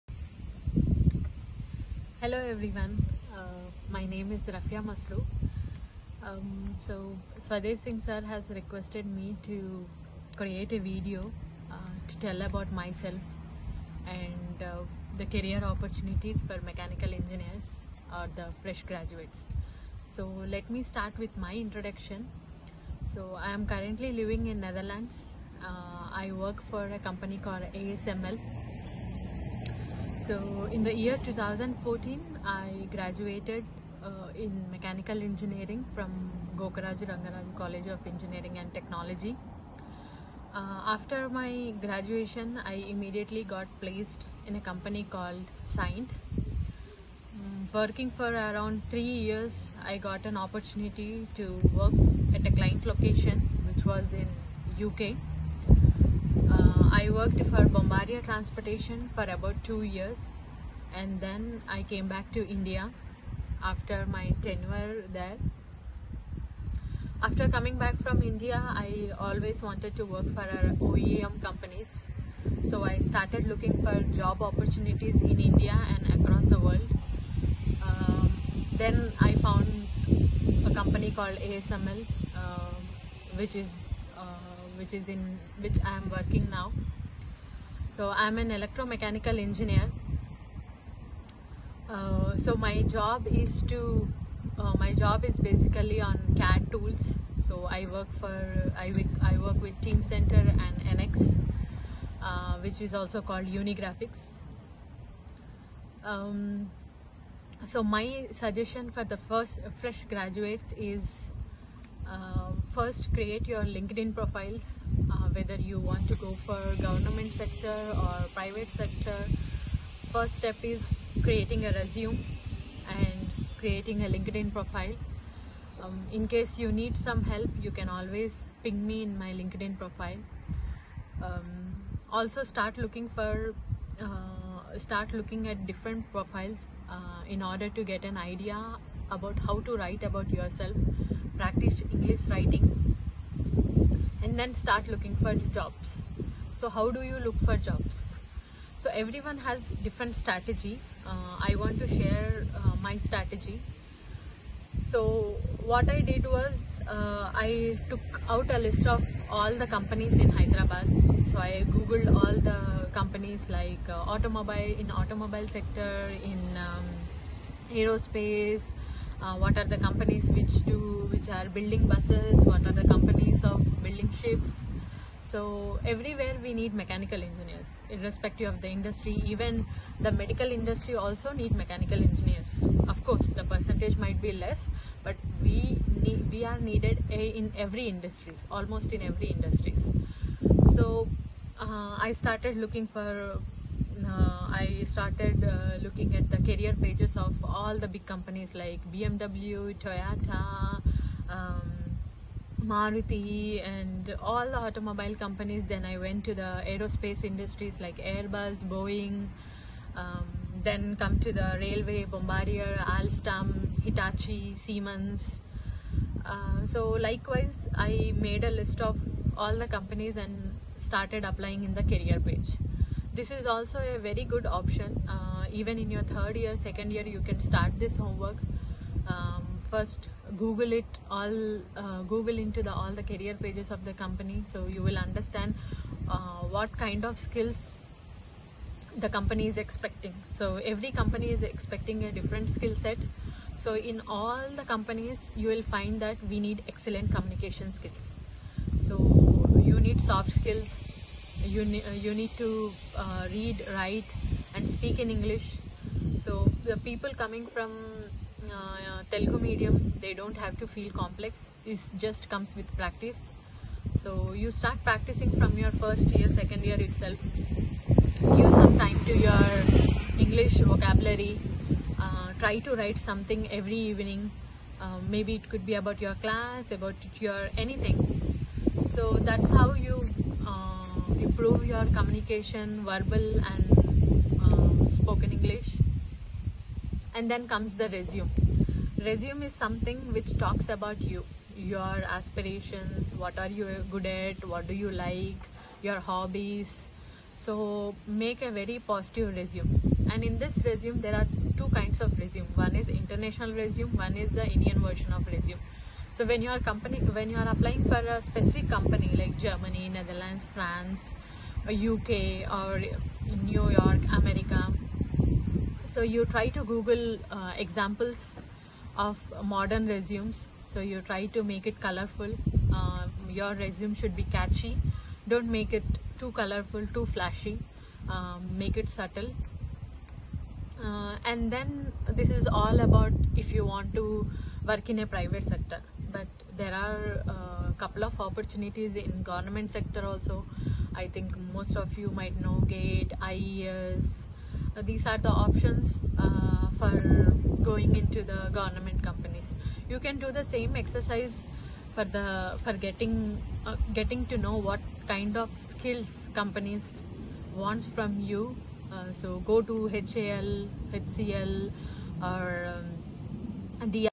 Practical tips to find core jobs in Mechanical Engineering
Hear from one of my bright student. What you should do to get core jobs in Mechanical